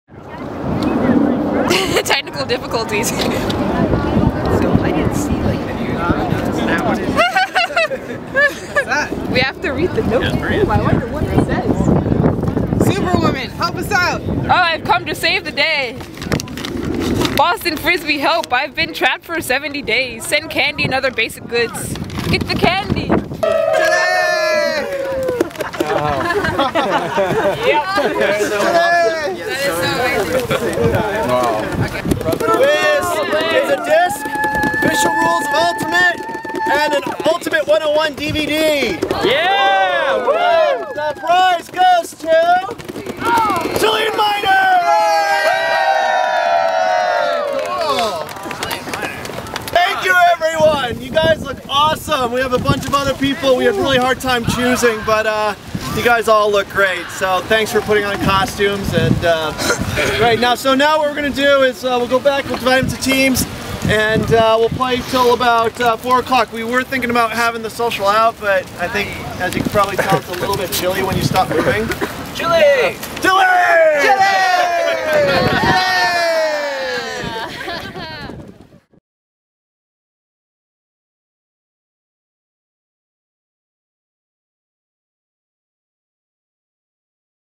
Boston Frisbee is Chile!
A windy and chilly day can't stop these freaky disc lovahs from having
a good time. And thanks to Jason, we can be comfortable with Chile!